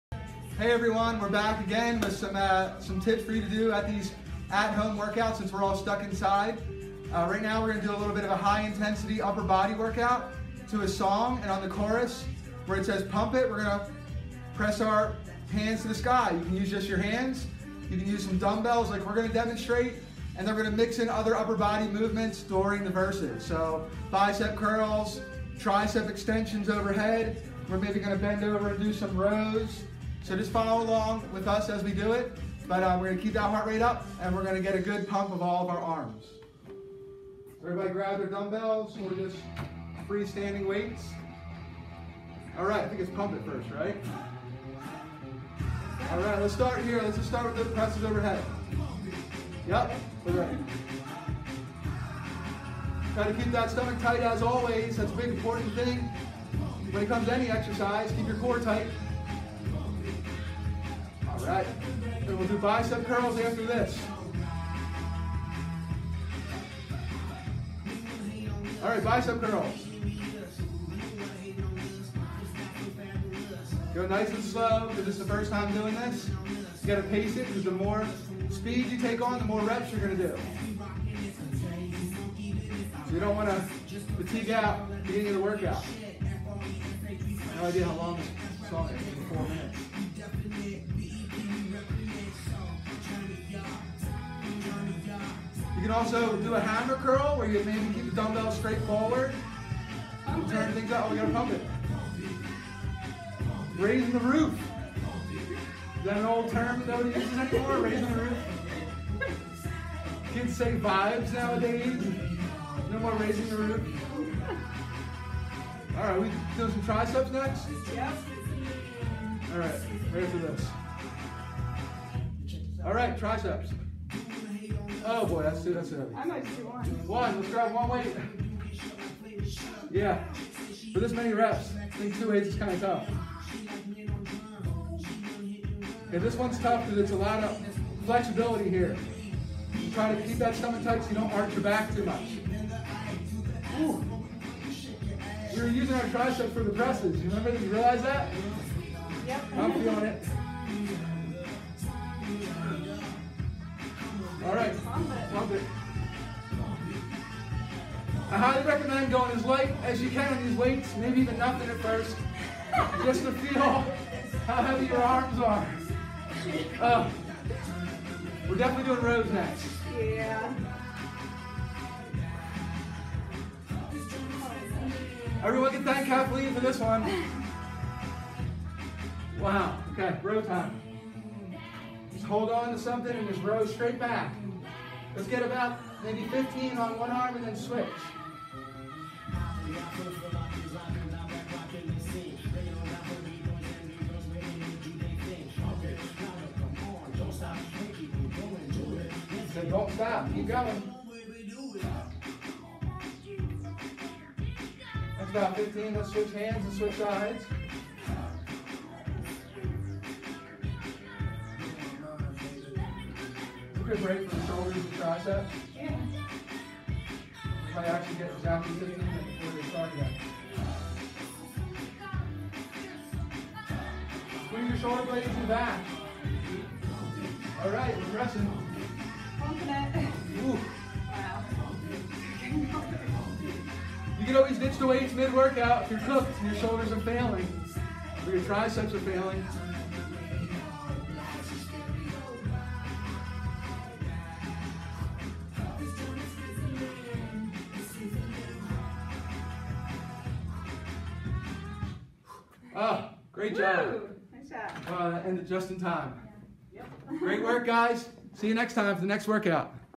Upper body burner a home!
You can do this workout program at home with zero equipment.